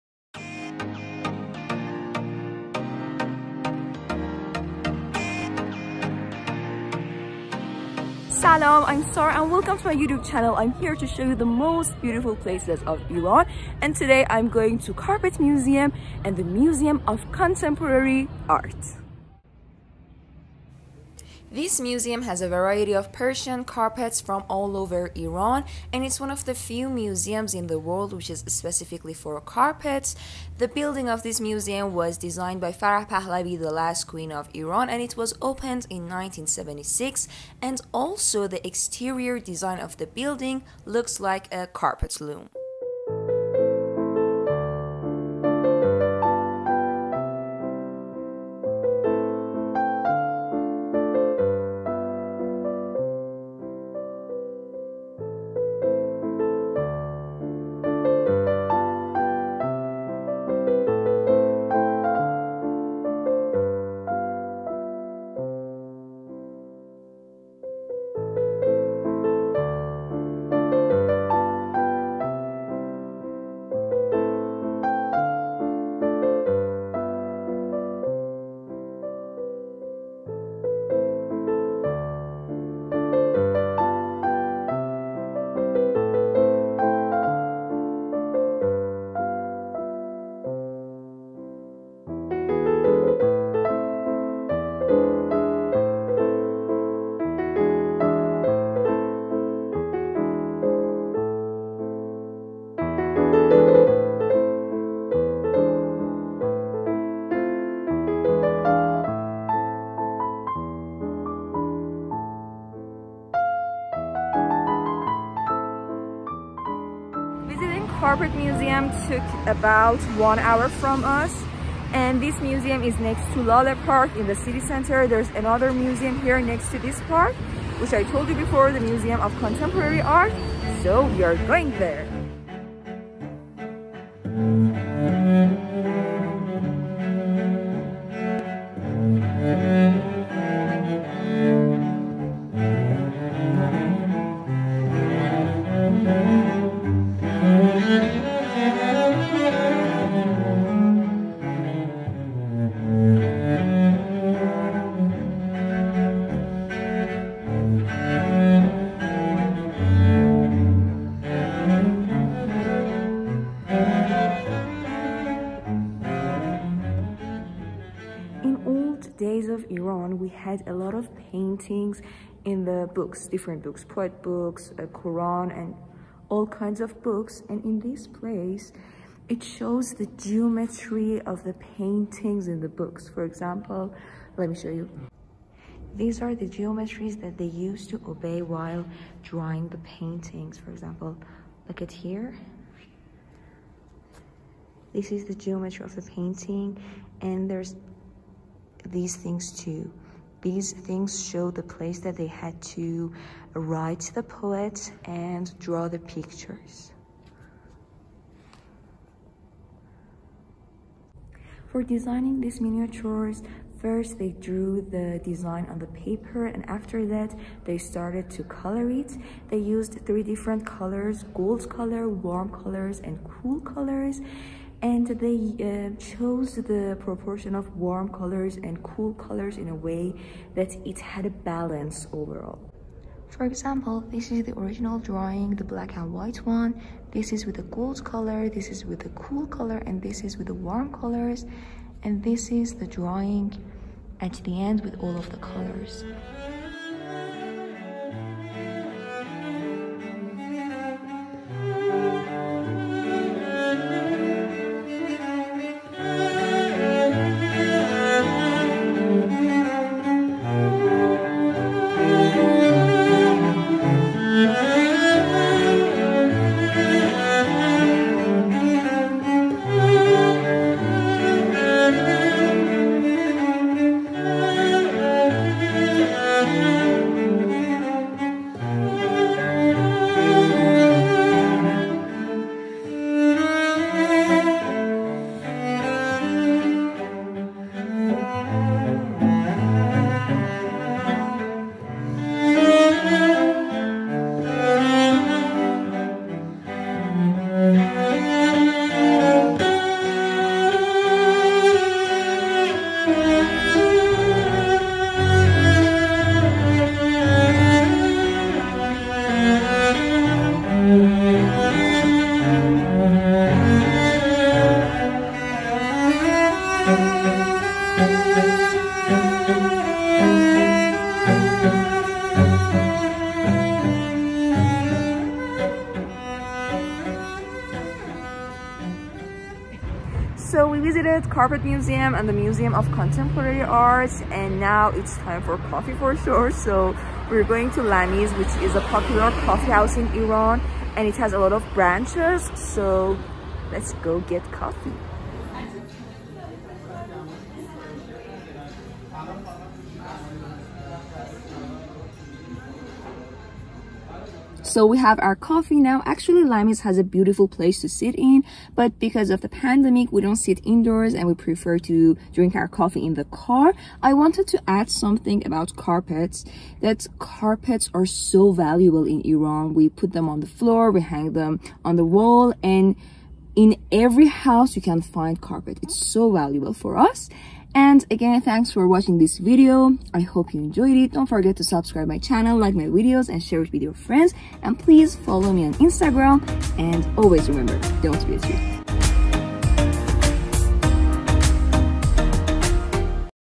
Carpet Museum & The Museum of Contemporary Art | موزه فرش و موزه هنرهای معاصر
The Tehran Carpet Museum of Iran is one of the world’s few museums that specializes only in carpets. Persian carpets are well known all around the world, and this museum offers the rare and exciting opportunity to visit and appreciate them in their country of origin. Tehran Museum of Contemporary Art is among the largest art museums in Iran. It has one of the greatest collections of Iranian modern and contemporary art. So join me, Sara Kalantari, in "don't be a tree" to explore some of the museum’s history and its greatest treasures.

موزه فرش تهران یکی از معدود موزه‌های جهان است که فقط در زمینه فرش فعالیت می‌کند. فرش ایرانی در سرتاسر جهان شناخته شده است و این موزه فرصت کمیاب و هیجان‌انگیزی را برای بازدید و قدردانی از آنها در کشور مبدأ خود فراهم می‌کند. موزه هنرهای معاصر تهران یکی از بزرگترین موزه‌های هنر ایران است. یکی از بزرگترین مجموعه‌های هنر مدرن و معاصر ایران را در اختیار دارد. پس با من، سارا کلانتری، در «درخت نباشیم» همراه باشید تا از این موزه‌ها دیدن کنیم